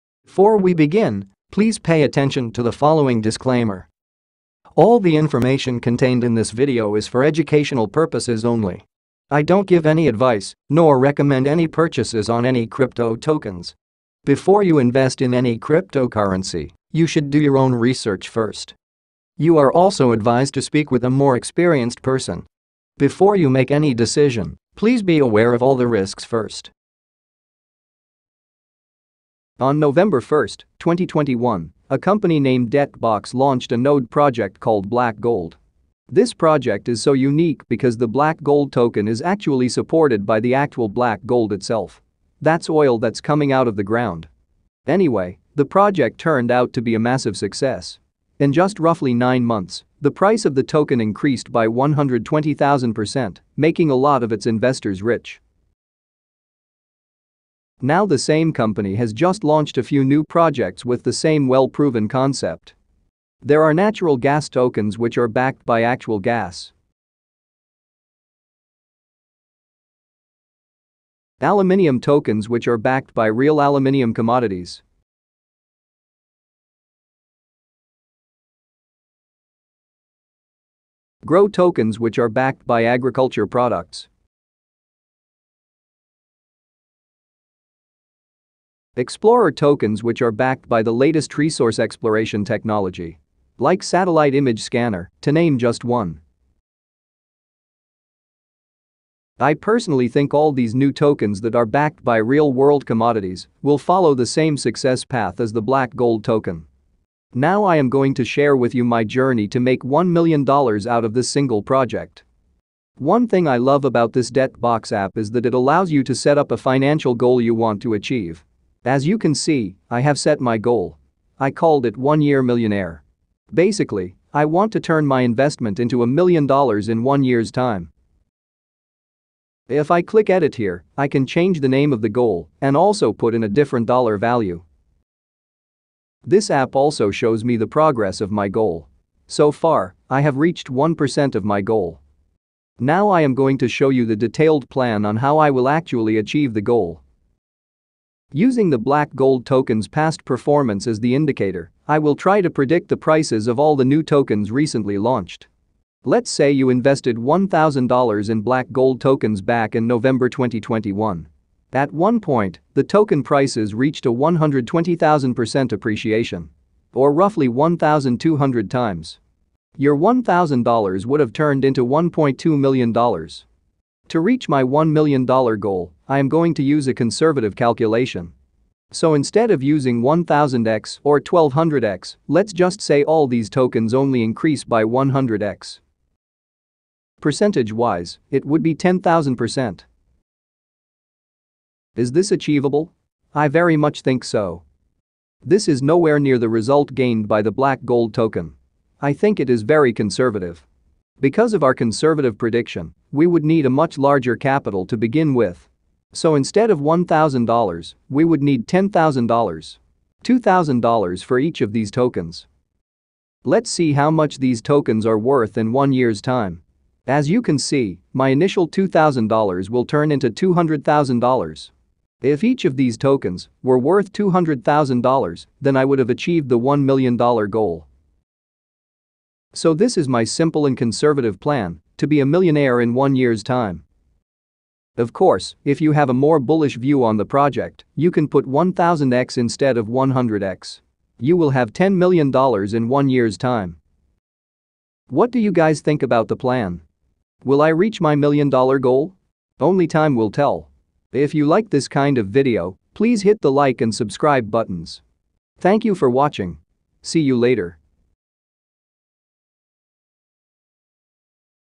My Plan To Make A Million Dollars In A Year Relying On Project X Node From Debt Box Company
In this video, I share my detailed plan for making a million dollars in a year using the Debt Box Company's Project X Node.

Other Video References Regarding the Project